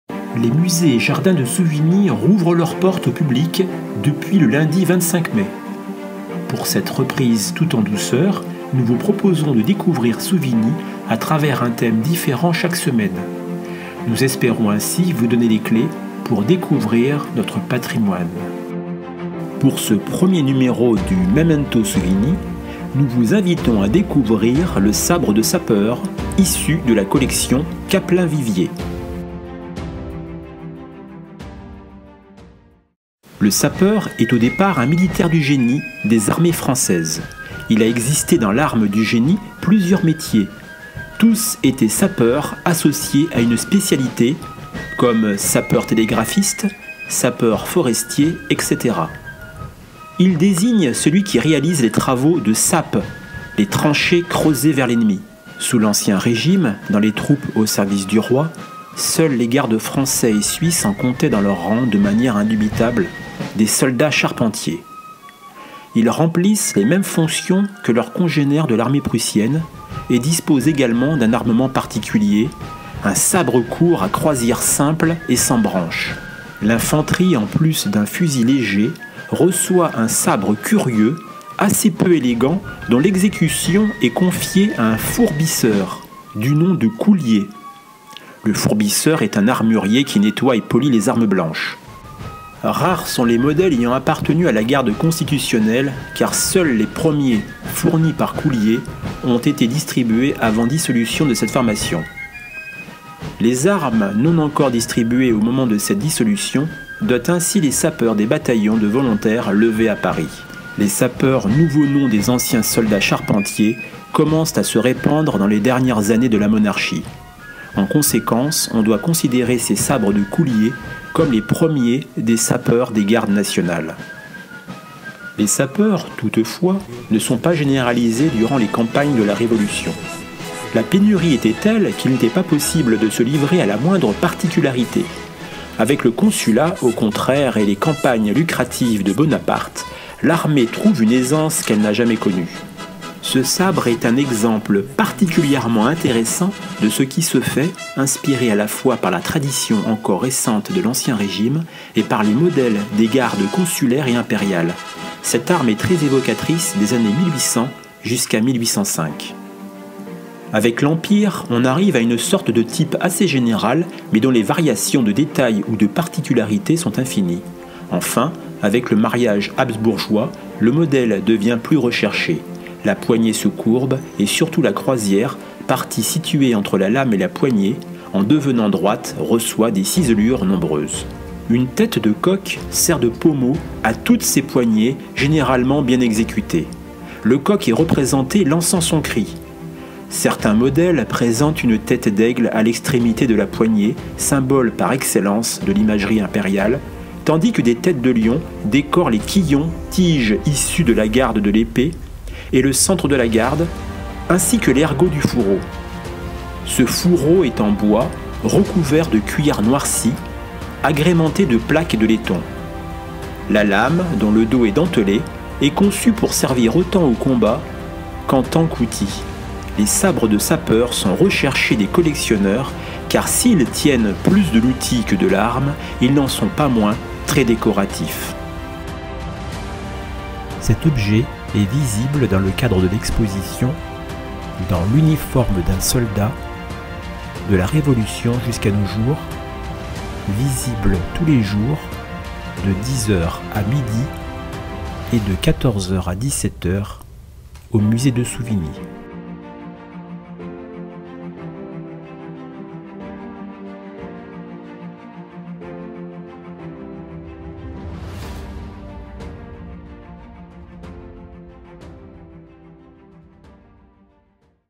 Memento Souvigny N°1
Les musée et jardin de Souvigny, rouvrent leurs portes au public depuis le lundi 25 mai 2020. Pour cette reprise toute en douceur, nous vous proposons de découvrir Souvigny à travers un thème différent chaque semaine. Nous espérons ainsi vous donner les clefs, pour découvrir notre patrimoine.

Pour ce premier numéro, nous vous présentons le sabre de sapeur, de la collection Capelin-Vivier, visible au musée et jardin du Prieuré, dans le cadre de l’exposition « Dans l’uniforme d’un soldat, de 1789 jusqu’à nos jours ».